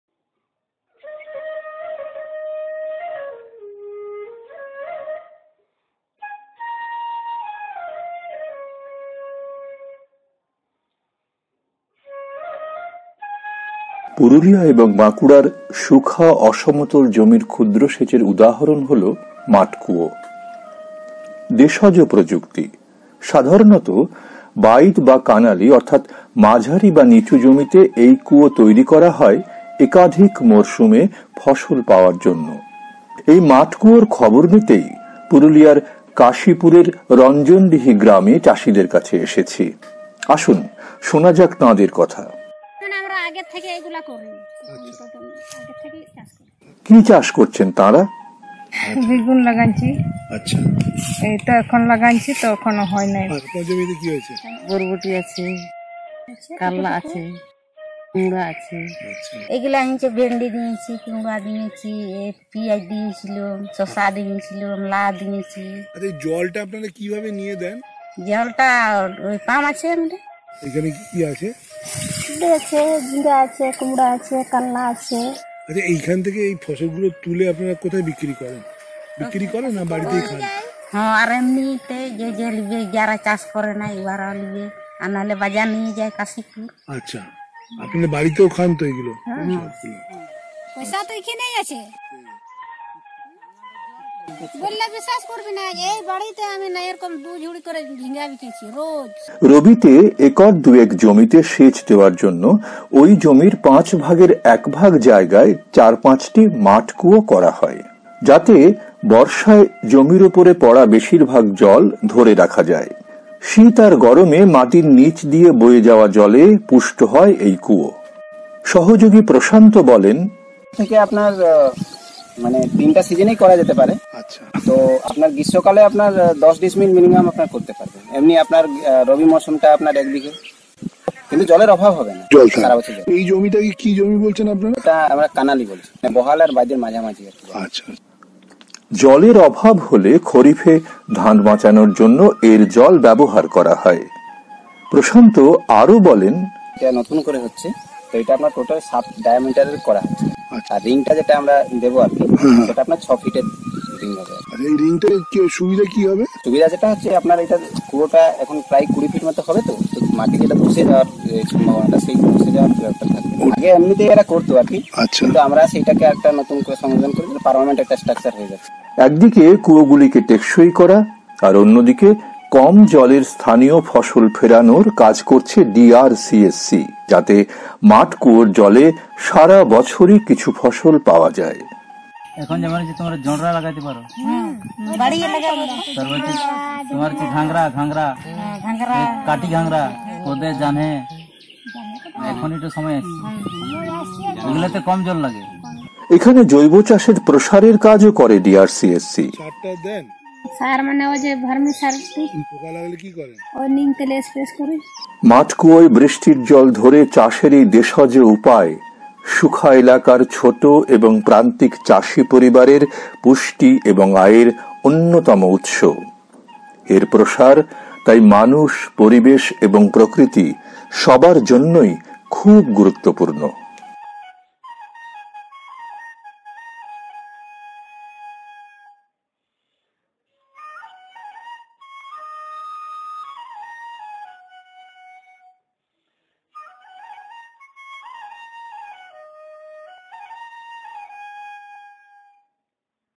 সেচের জন্য মাঠ কুয়ো Field Well for Irrigation.
শুখা অসমতল জমিতে বৃষ্টির জল ধরার আধার হল মাঠ কুয়ো। দেশজ প্রযুক্তি। সাধারণত মাঝারি বা নিচু জমিতে এই কুয়ো তৈরি করা হয় একাধিক মরশুমে ফসলে সেচ দেওয়ার জন্য।
Field well is an example of indigenous rainwater harvesting structure in undulating dry land. This well is usually dug in middle or lowland for year round irrigation.